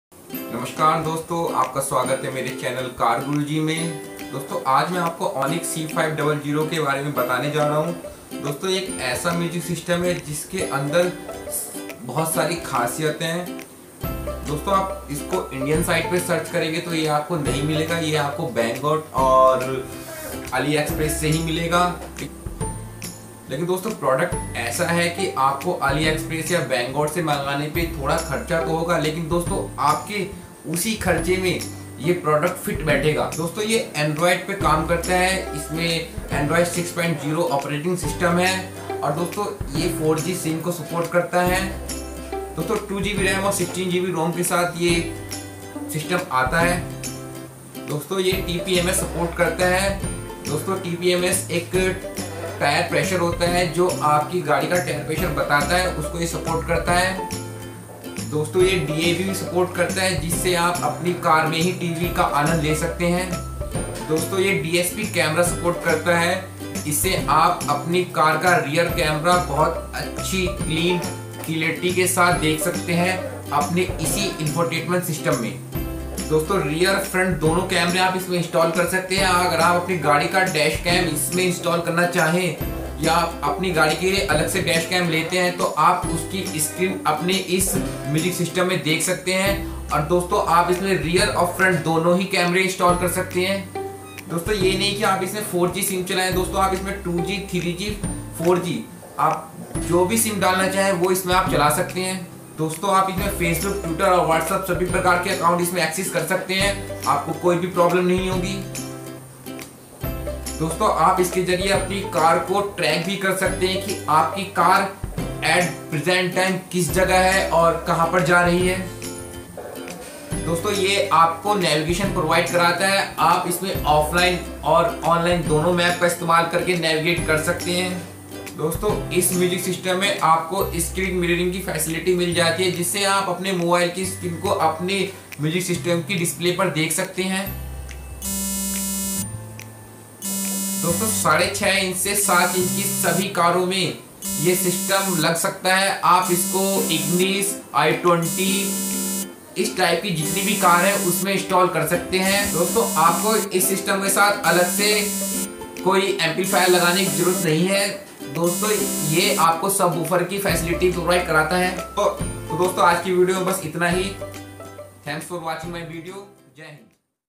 car music system in hindi
Dosto ye video ownic c500 ki jankari batayi hai ise se acha music system khi nhi milega.